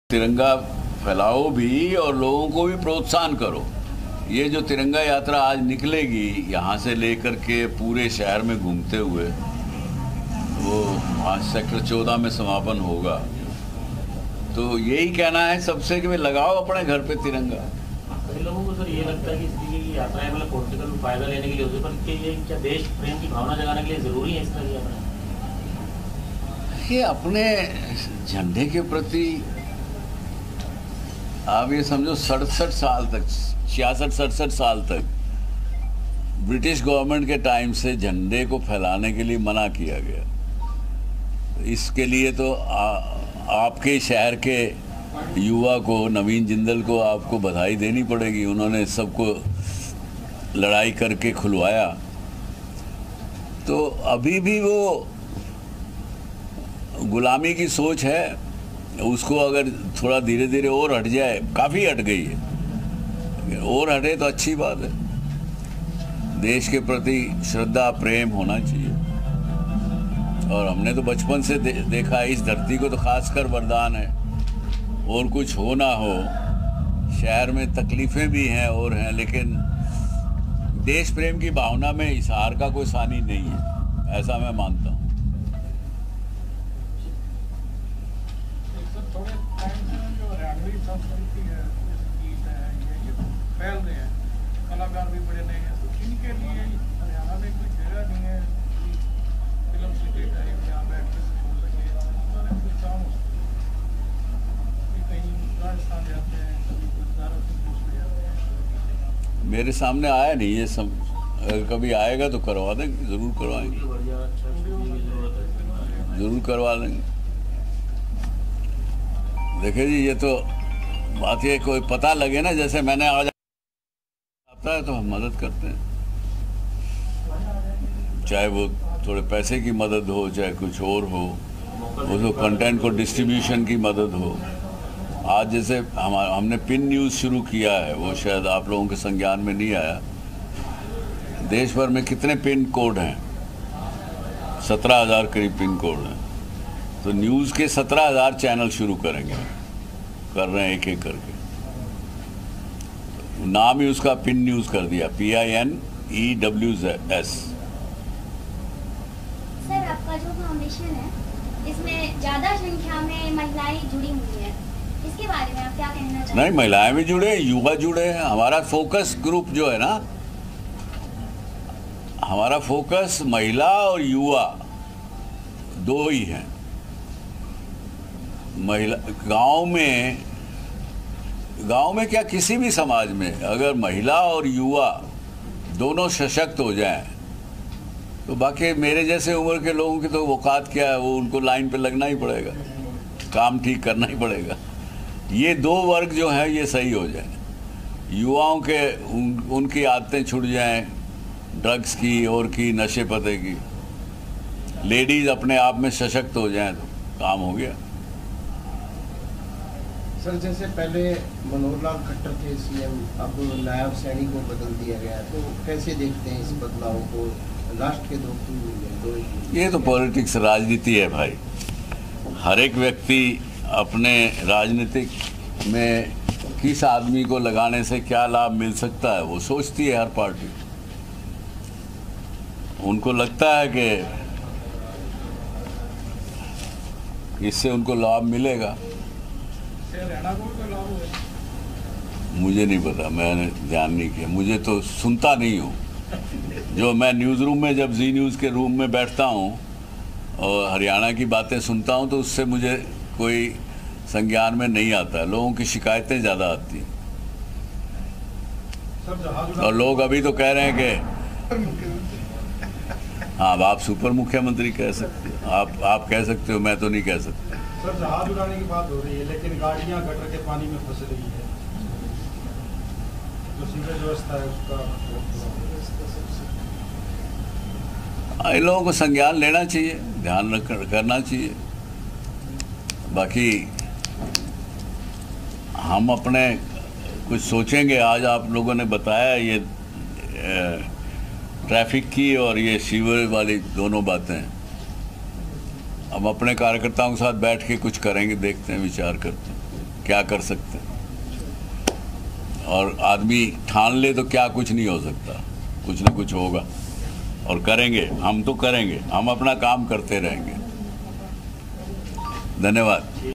Independence Day 2024: पूर्व राज्यसभा सांसद डॉ. सुभाष चंद्रा का संदेश | Har Ghar Trianga | Yatra
Independence Day 2024: देशभर में तिरंगा यात्रा का आयोजन, हिसार में निकाली जाएगी तिरंगा यात्रा, पूर्व राज्यसभा सांसद डॉ. सुभाष चंद्रा का संदेश, हर घर पर तिरंगा लगाएं-डॉ. सुभाष चंद्रा

Independence Day 2024: Tiranga Yatra organized across the country, Tiranga Yatra will be taken out in Hisar, message from former Rajya Sabha MP Dr. Subhash Chandra, put tricolor on every house - Dr. Subhash Chandra